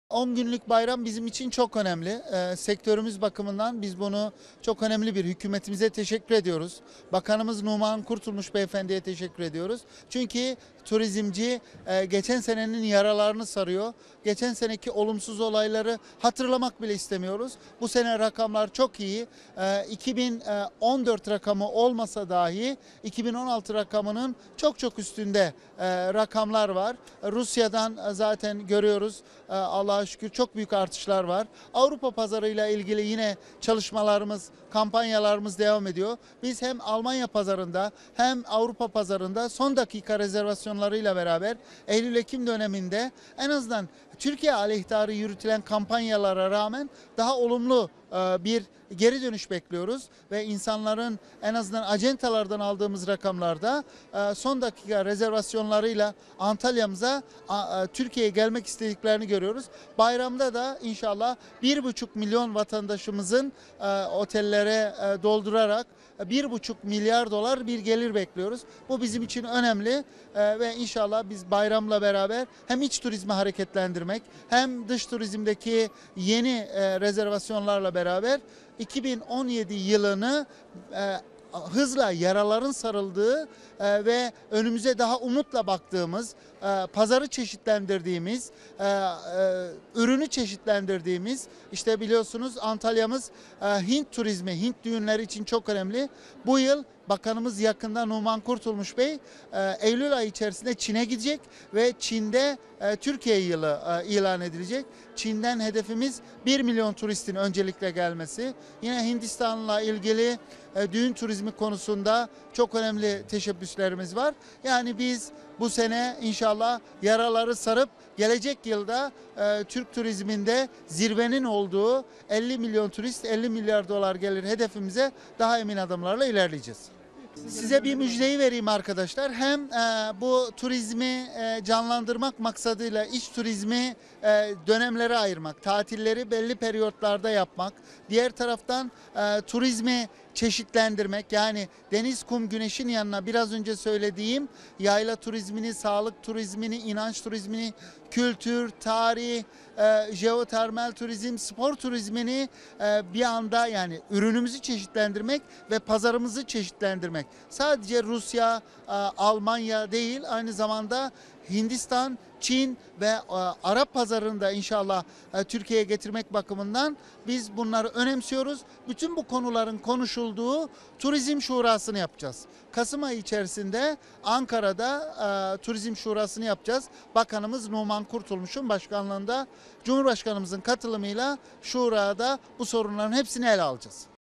​Kültür ve Turizm Bakan Yardımcısı Yayman: "1,5 milyon yerli turistten 1,5 milyar dolar bekliyoruz"
Kültür ve Turizm Bakan Yardımcısı Hüseyin Yayman, 10 günlük bayram tatili süresince otelleri dolduracak 1,5 milyon yerli turistten 1,5 milyar dolar gelir beklendiğini söyledi.